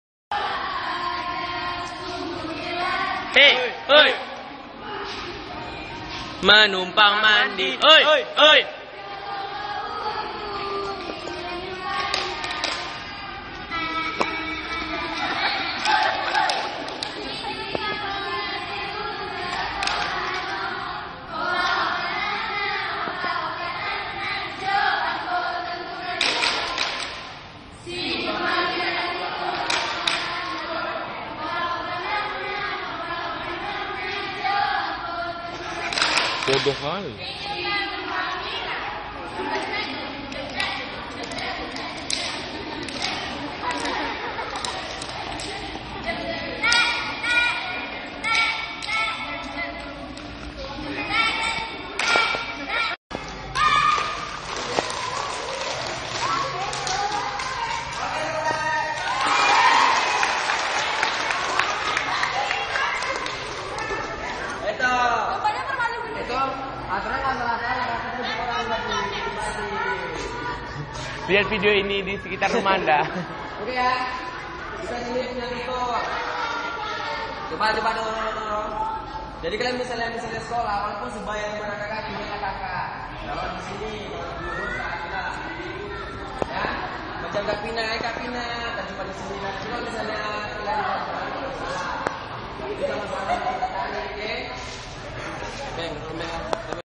TAri sAMAn SMA DhArMA pAtrA.mp4
lAtihAn tARi sAMAn dIsEkolAh sMA DHArMA pAtRA dALAm EKStRAkulikUler sEndrAtARi